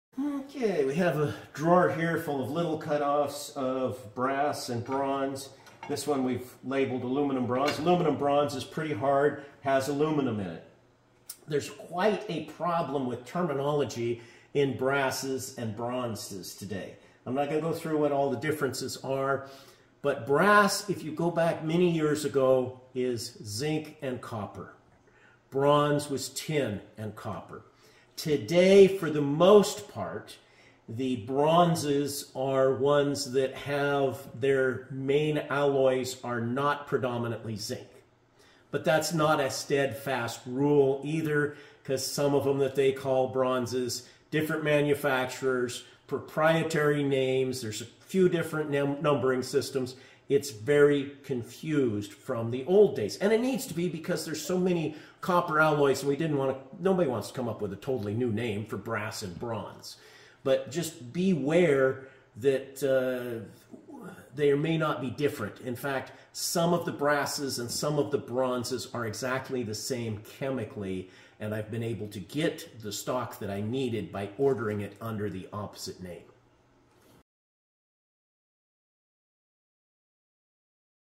Machinist's Minute(s): The Difference Between Brass and Bronze
Brass and bronze are two different things, except for when it's not.